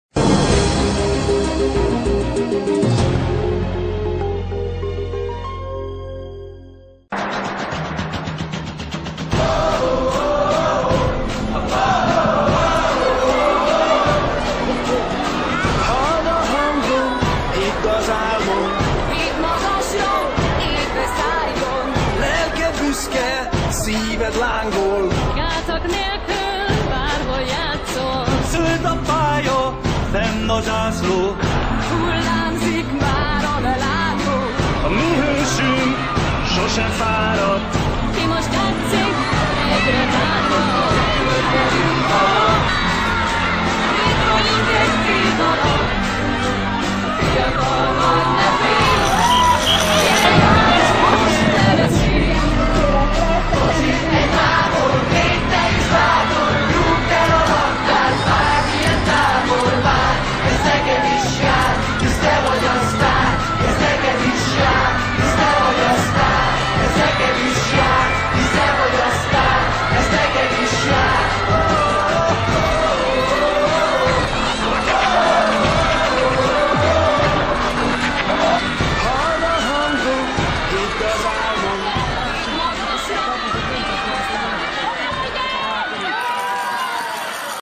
Százszámra potyognak a gólok a nyíregyházi stadionban - szon.hu
A Bozsik programban a város több, mint 20 óvodájából 600 gyerek vesz részt. Nekik szerveztek pénteken egy nagy gálát a Városi Stadionban. Videó: Pusztai Sándor